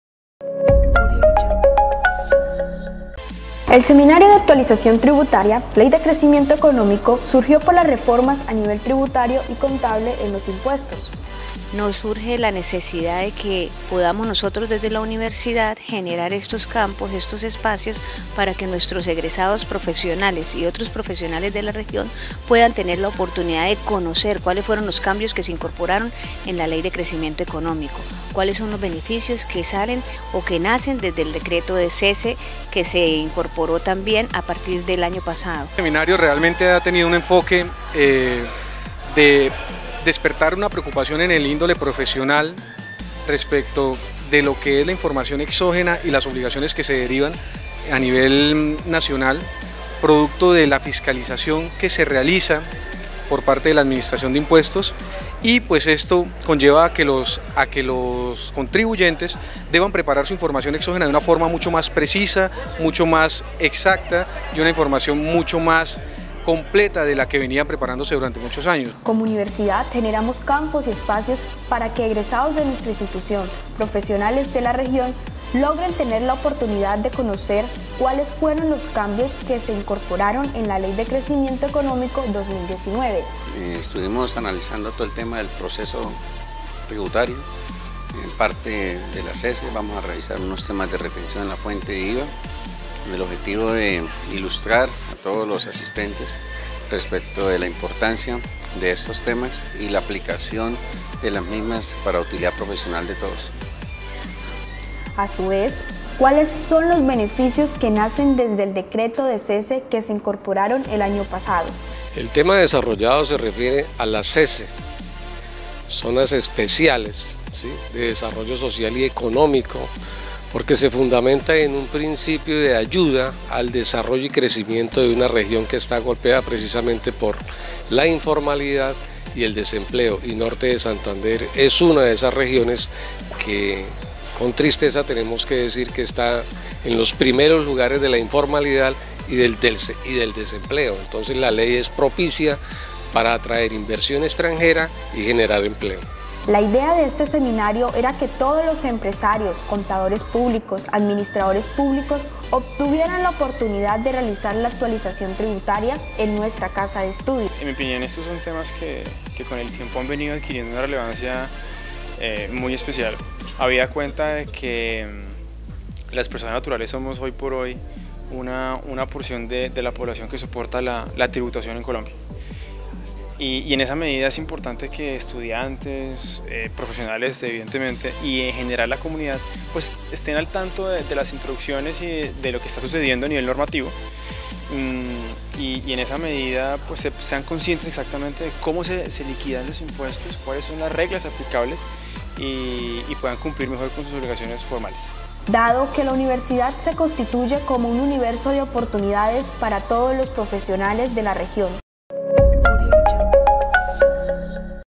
Nota Informativa - Seminario en Actualización Tributaria
La Universidad Libre Seccional Cúcuta desarrolló un seminario en actualización tributaria para estudiantes y profesional de la región.